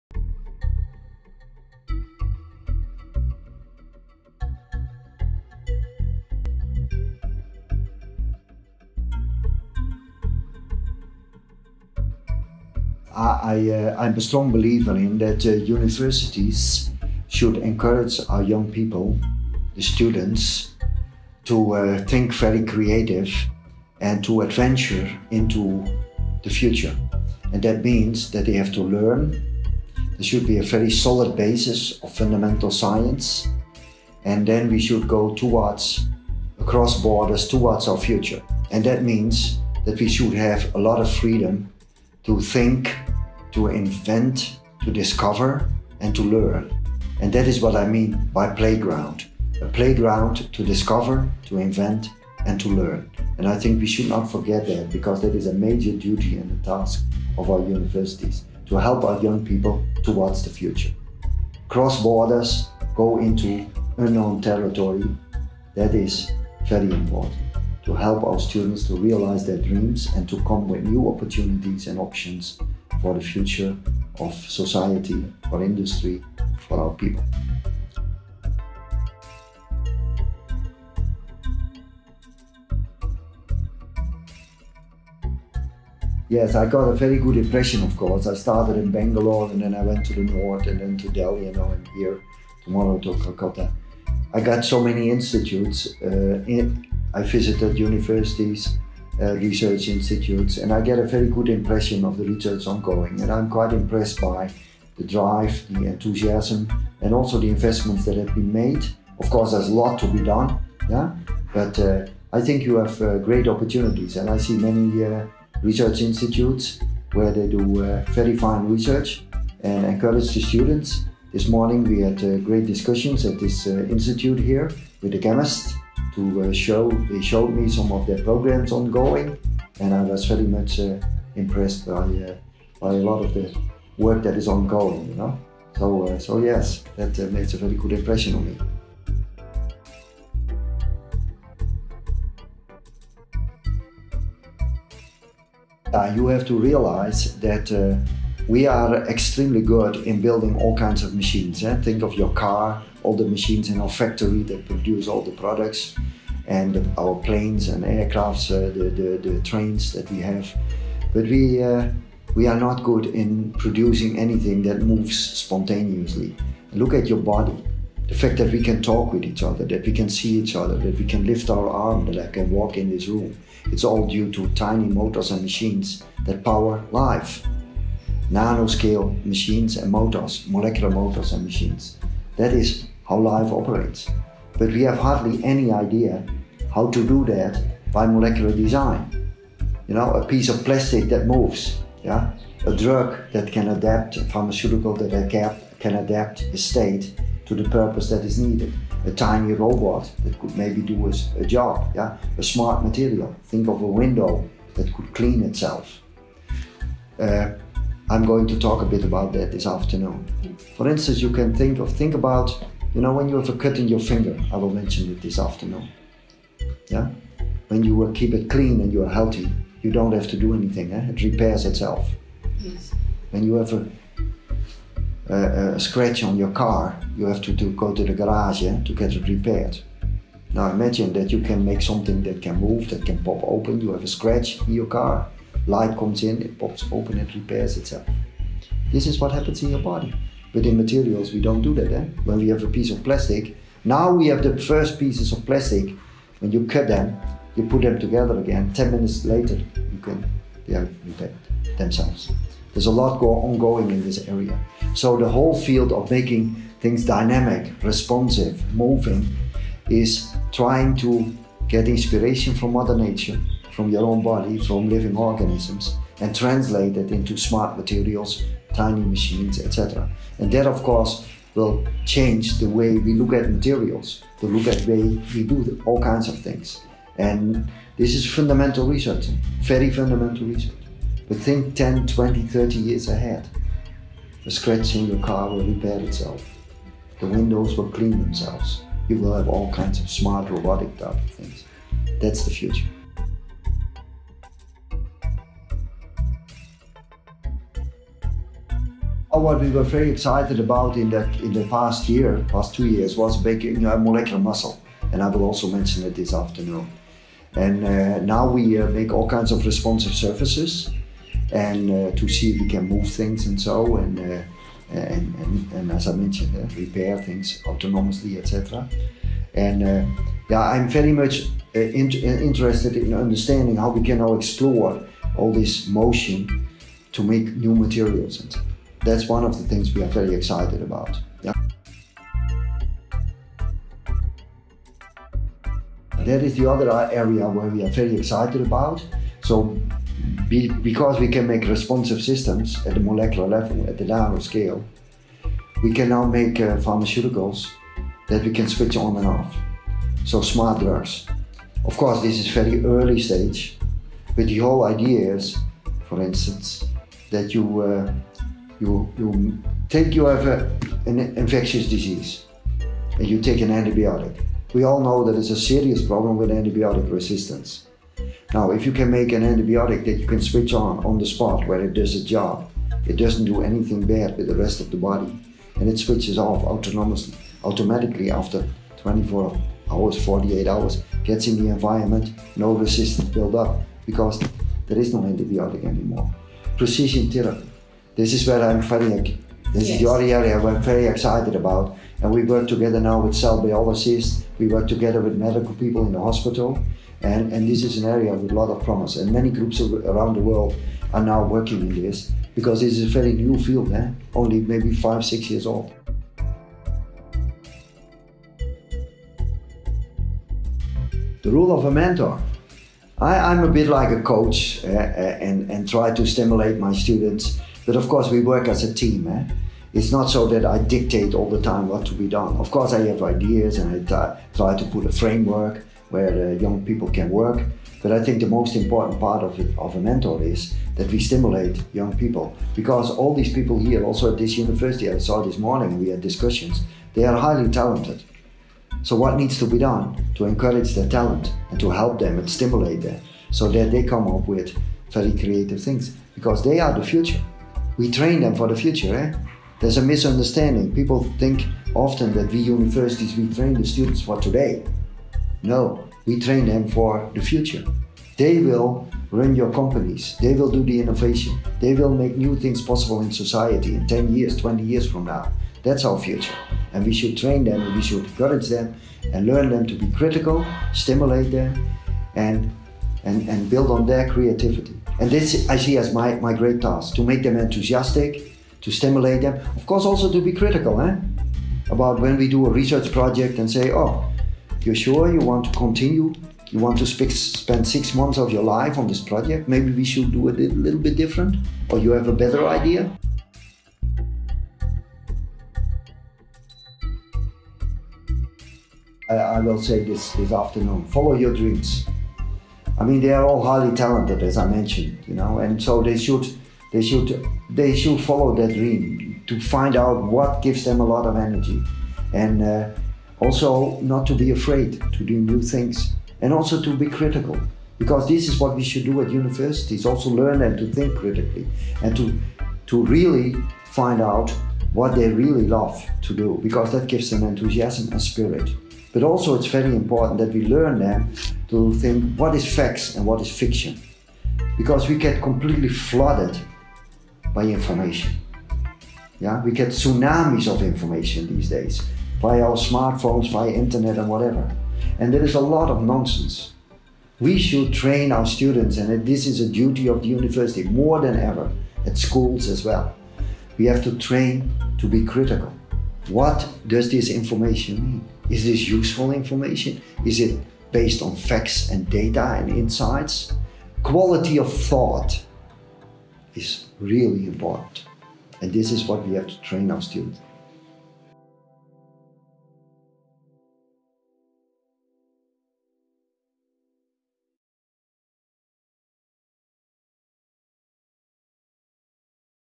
Interview Prof. Ben Feringa at IIT Kharagpur
“The duty of universities, more than ever now, is to train students to think critically,” ~ Professor Ben L. Feringa Nobel Prize awardee for Chemistry, 2016